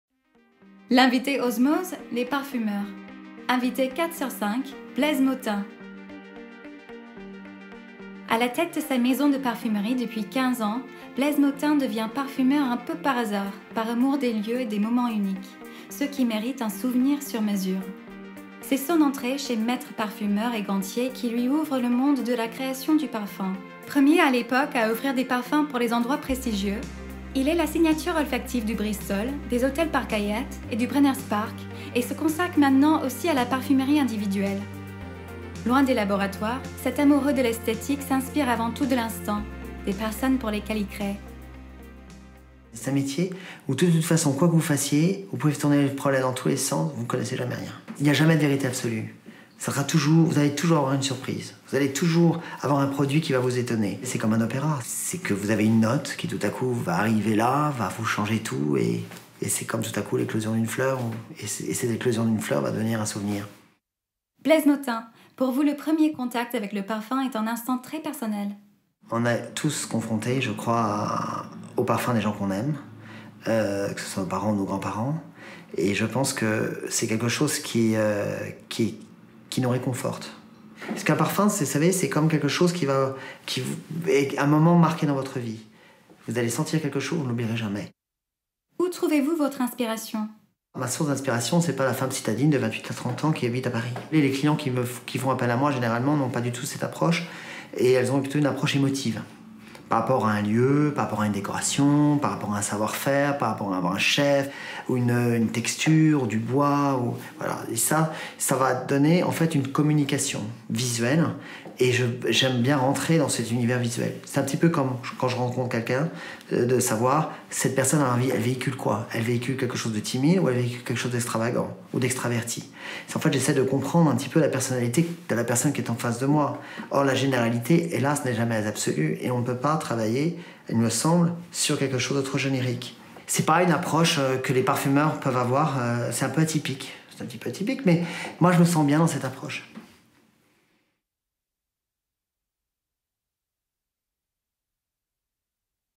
OSMOZ TV Invité 4 sur 5 Blaise Mautin (fr)
Entretien  avec le parfumeur Blaise Mautin, créateur de parfums sur mesure qui préfère travailler loin des laboratoires et au plus près des personnes qui viennent à lui. Amoureux depuis toujours des belles histoires, il les habille de parfums uniques.